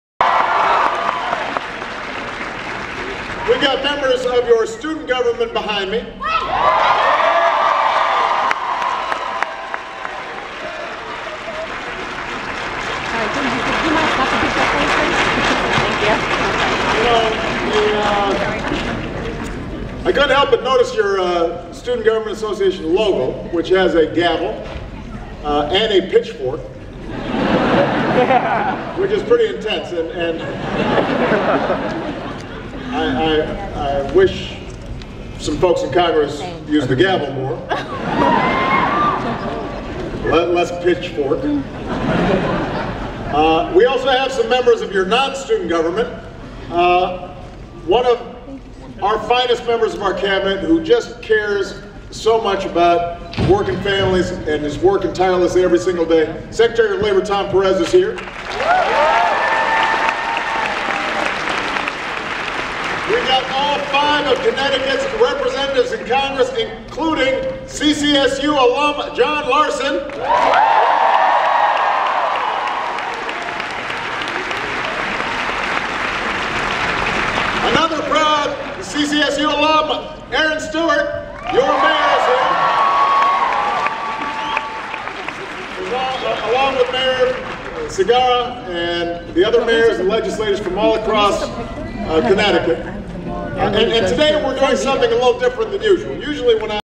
Barack Obama at Central Connecticut State University-Part 1
Filmed live by me at speech regarding potential increase in minimum wage. Not meant to infringe upon any news stations copyright, if it does please let me know and it will be removed.